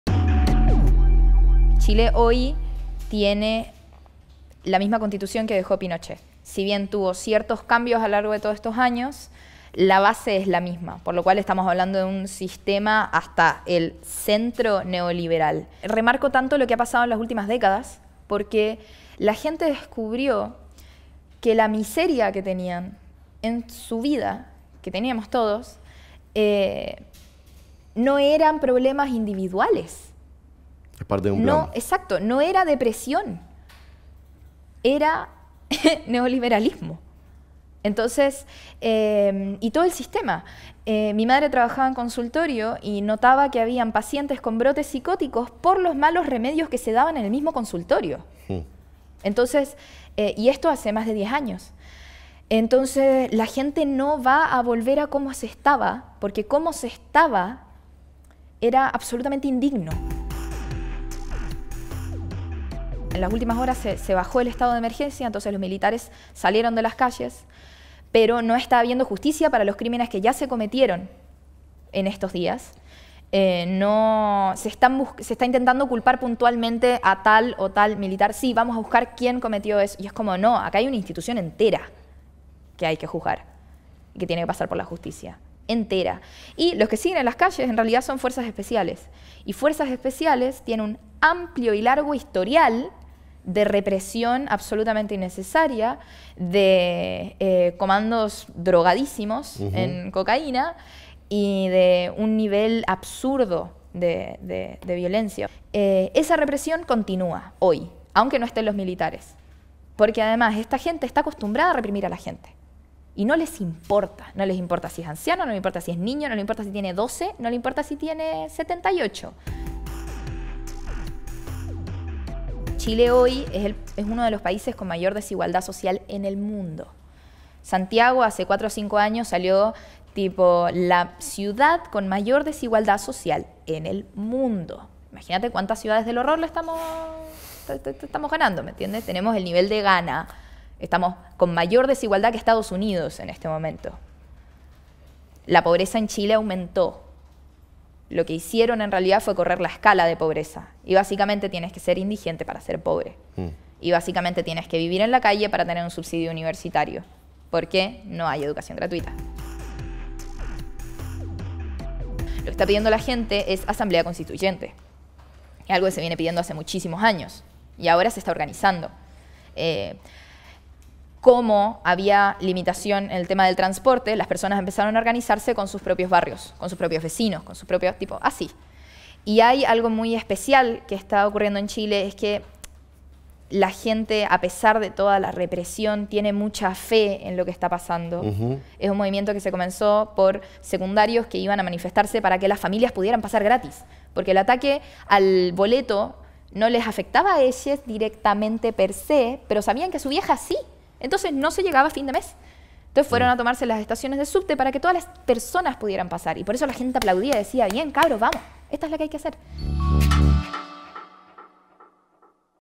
ChileDespertó | La visión de Carla Díaz Ferrera, residente en Argentina
Mientras el gobierno de SebastiánPiñera se esfuerza por esconder y desactivar el estallido social de los últimos días, miles de chilenos siguen en las calles en rechazo a un modelo neoliberal que profundiza la desigualdad. La visión de Carla Díaz Ferrera, de la Asamblea de Chilenxs en Argentina.